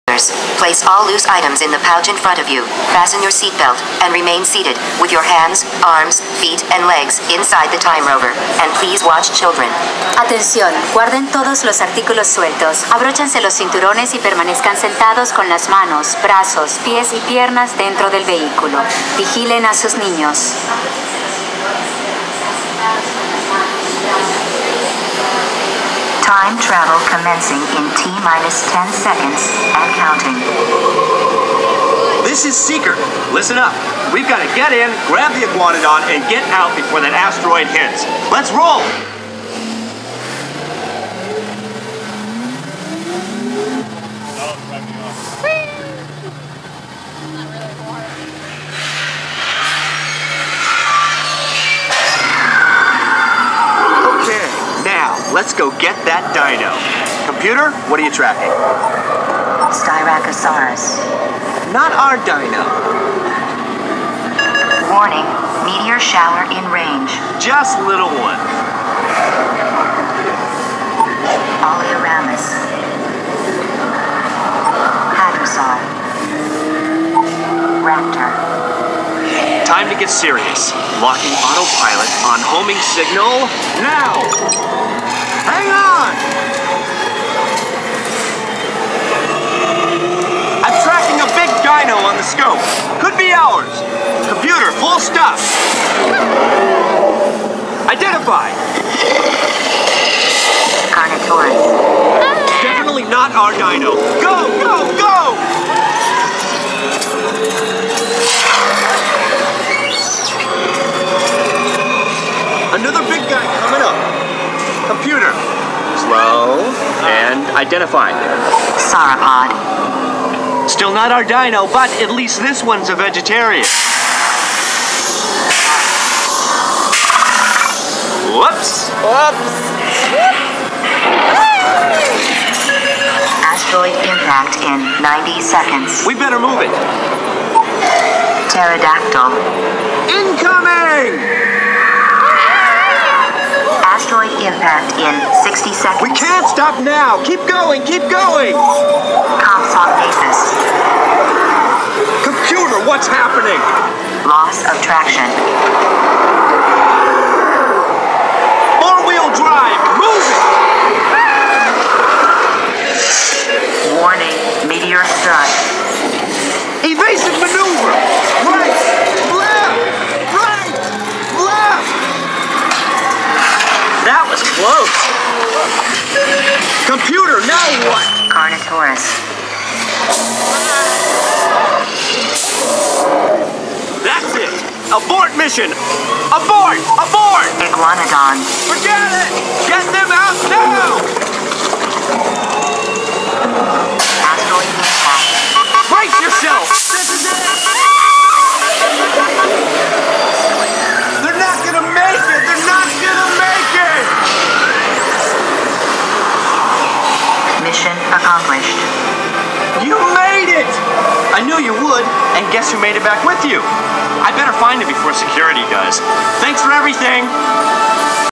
Dinosaur, Disney's Animal Kingdom Park, Walt Disney World
Go back in time to the dawn of the dinosaurs and try and bring home the dino. But look out for the meat eating vicious dinosaurs.